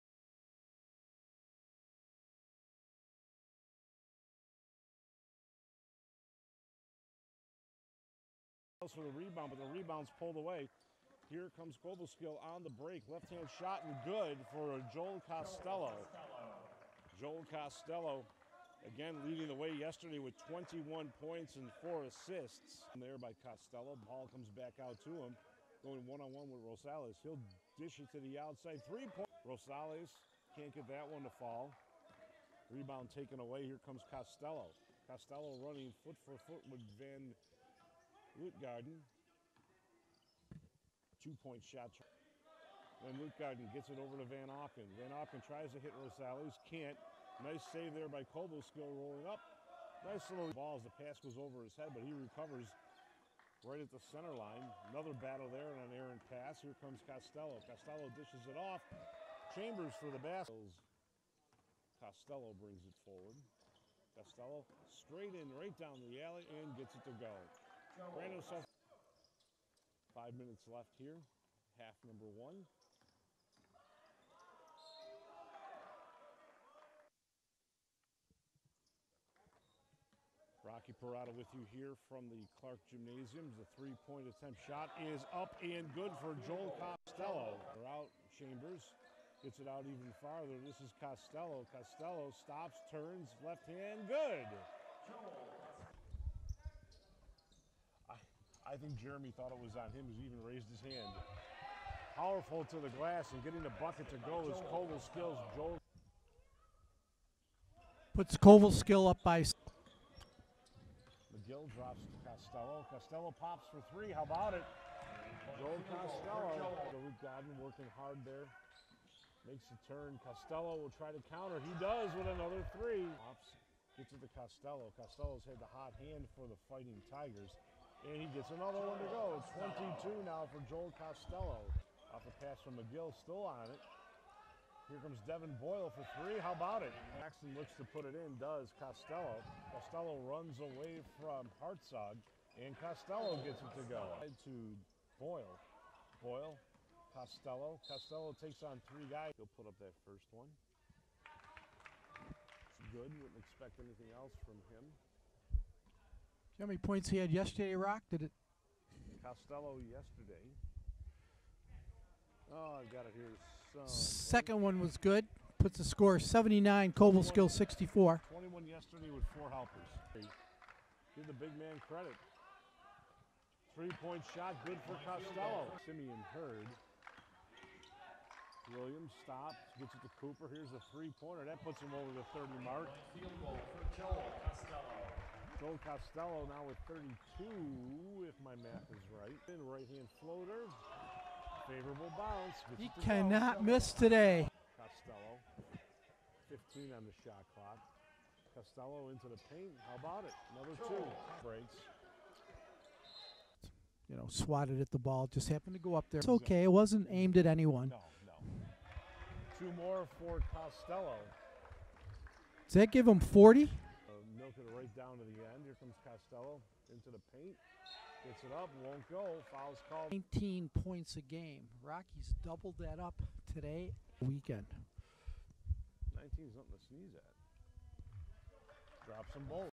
Joel Costello 42 Points vs. D'Youville College
42 Points            15-23 FG
6 Assists             4-6 3PT
5 Rebounds        8-9 FT
3 stls